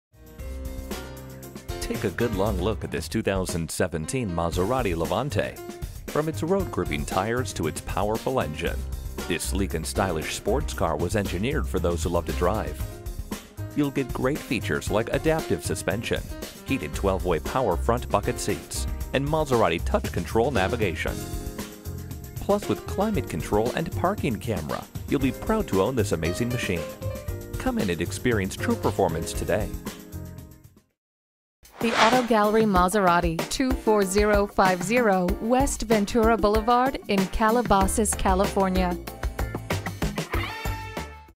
New 2017 Maserati Levante Los Angeles Calabasas, CA NMH231433 - SOLD
Year: 2017
Make: Maserati
Model: Levante
Engine: 3.0L V6
Transmission: ZF 8-Speed Automatic
Interior: Rosso/Nero
Stock #: NMH231433
VIN: ZN661XUS4HX231433

Address:
24050 W. Ventura Blvd.